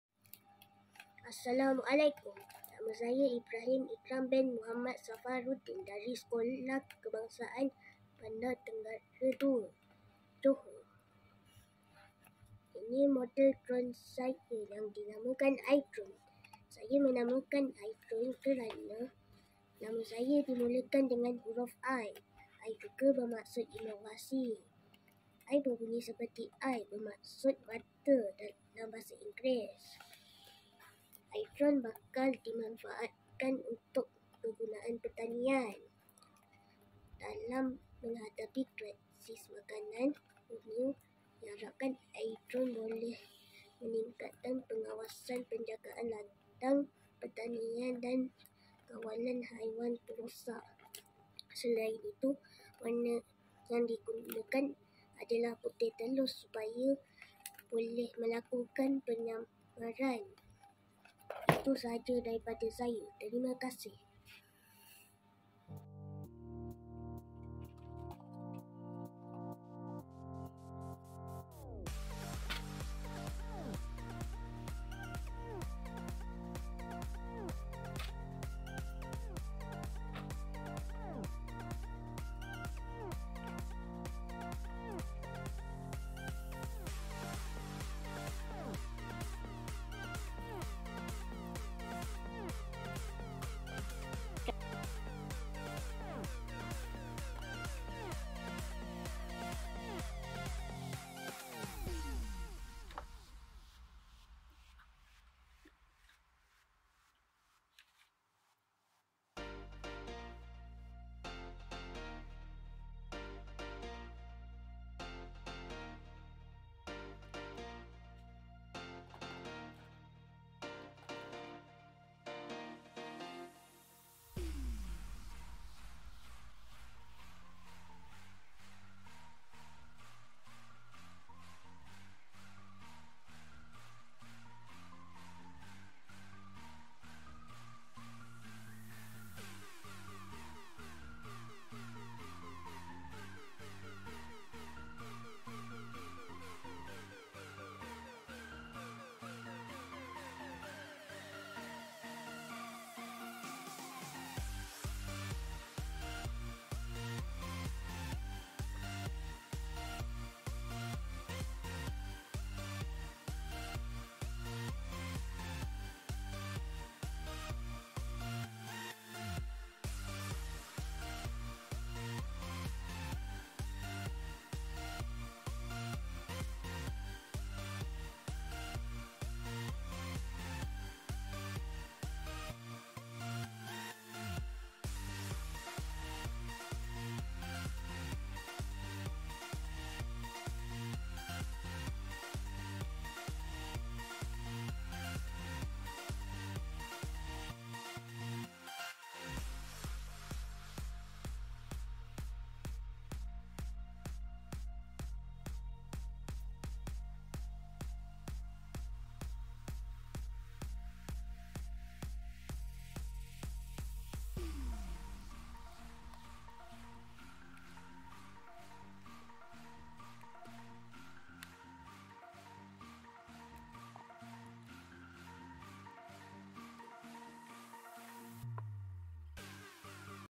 Junior Drone Edu Challenge 2022 - Ibrahim Ikram bin Mohammad Safarudin
DIY Drone Modelling Competition for Junior Drone Edu Challenge 2022 by Ibrahim Ikram bin Mohammad Safarudin.

I - drone